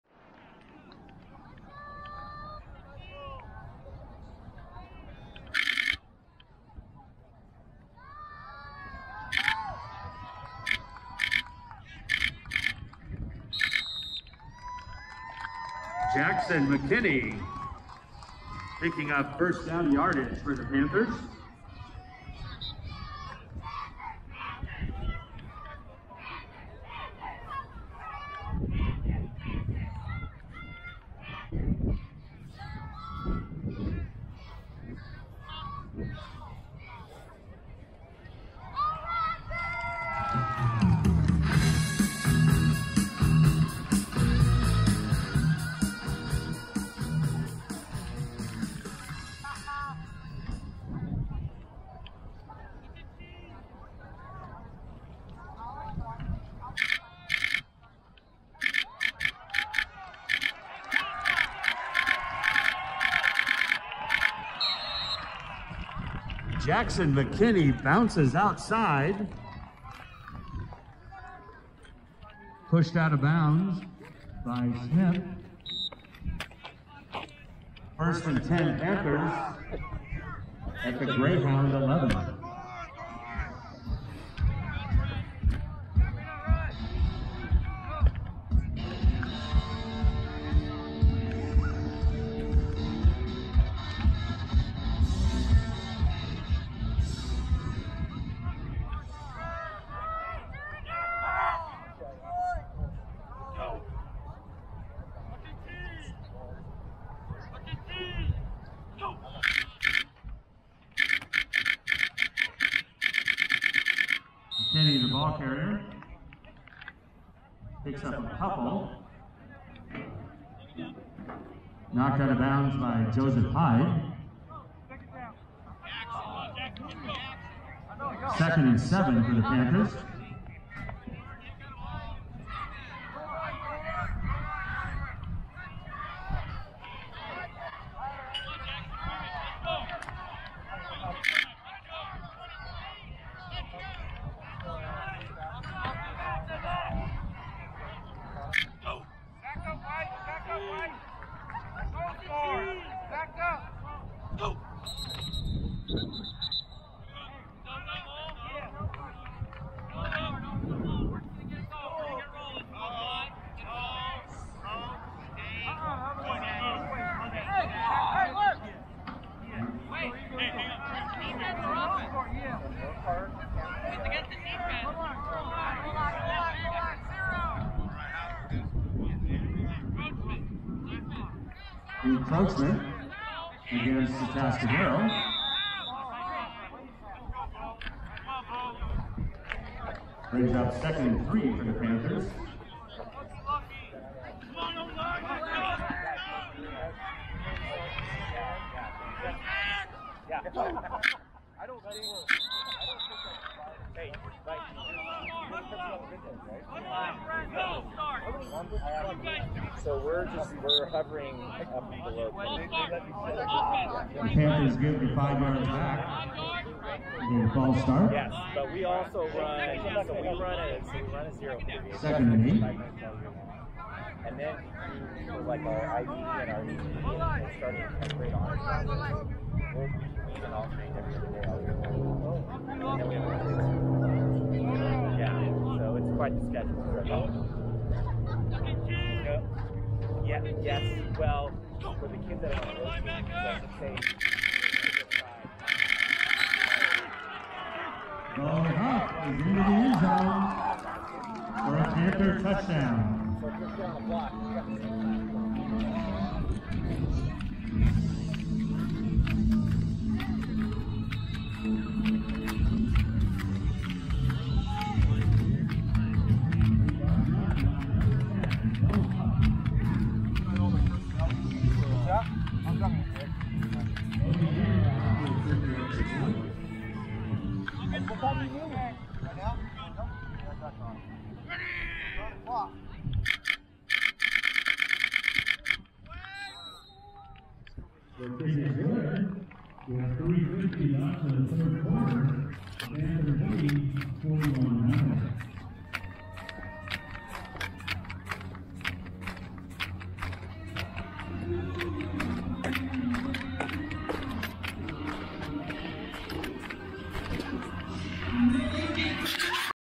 FPV: A Sports Photographer's View
Educational video for student sport photographers at Newbury Park High School, California, CA.